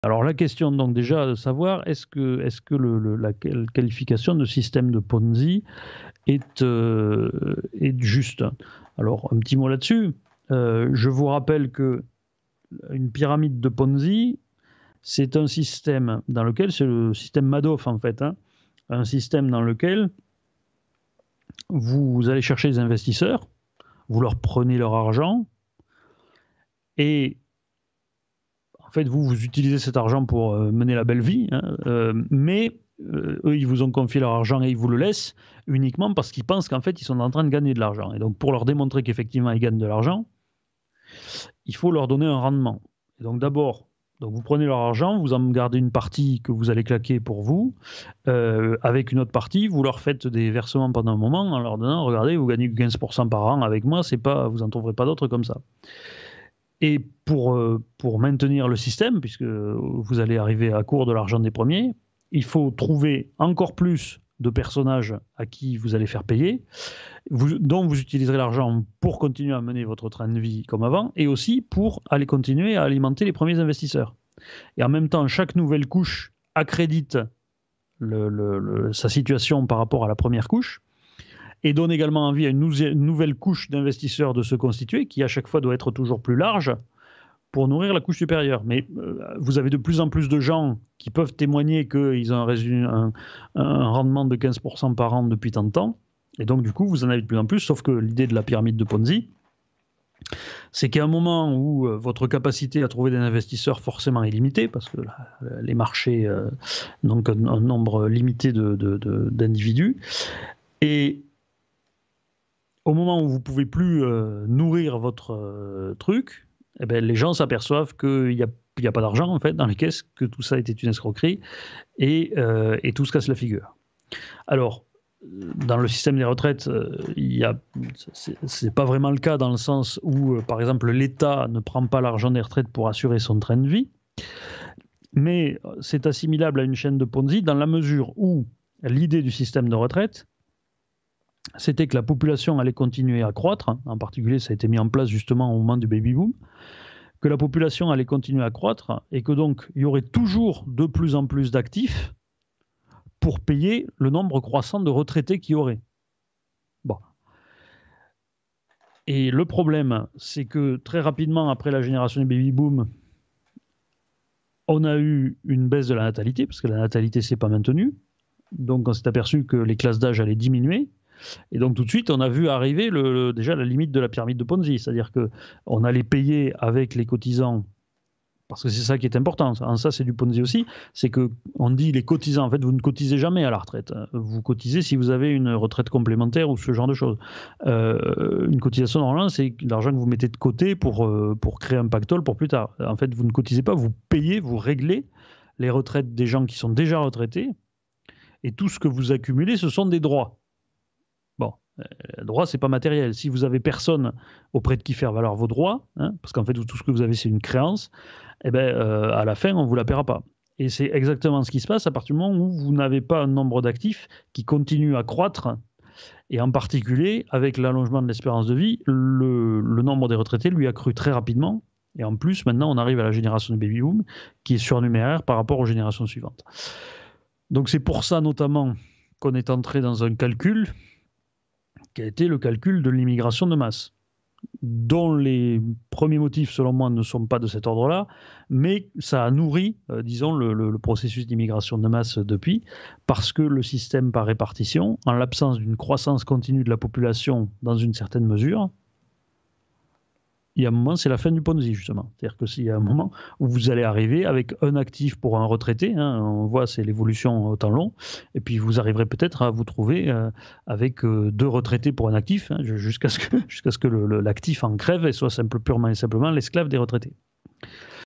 Est ce que les retraites sont un système de Ponzi ? - Philippe Fabry
Philippe Fabry à propos du système de retraite par répartition en France qui est un système de Ponzi.